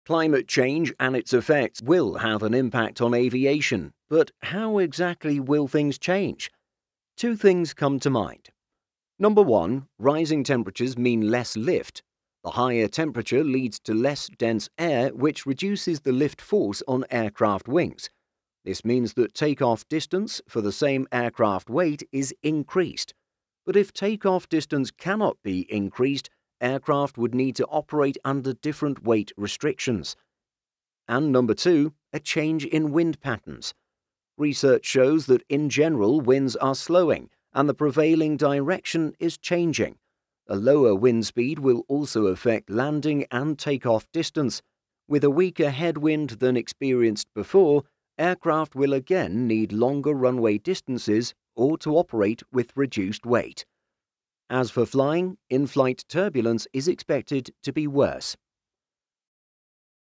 Will Rising Temperatures Impact Aircraft Takeoffs?
Climate change and its effects are hot topics for discussion! No pun intended.

This will have an impact on aviation, but how, exactly, will things change?